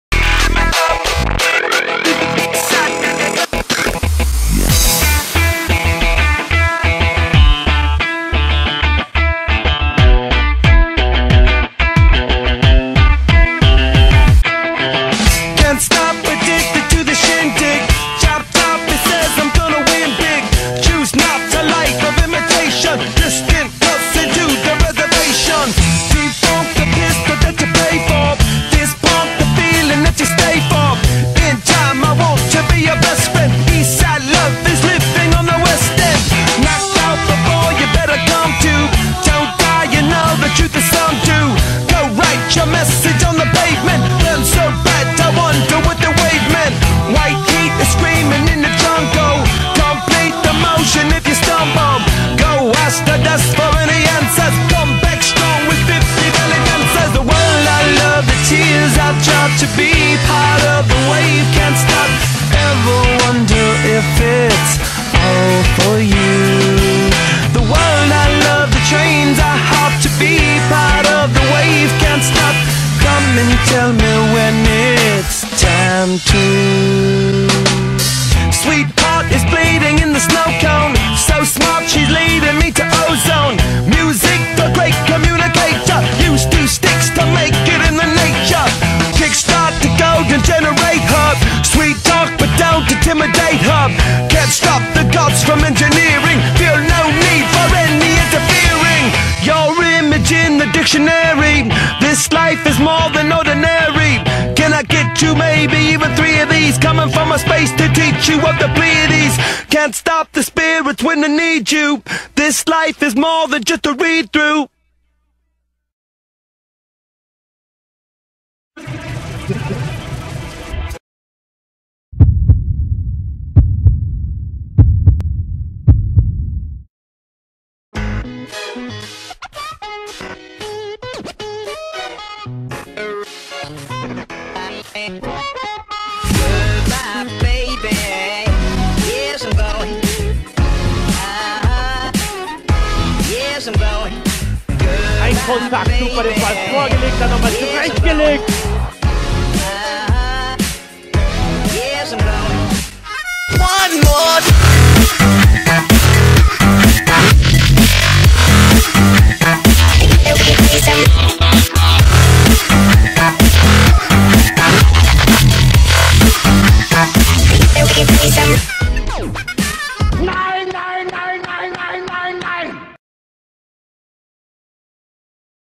Marco Reus - The Movie - 2009 bis 2013 - by ComReP
Hier ist ein neues Video über Marco Reus. Es zeigt wie er sich in den Jahren entwickelt hat.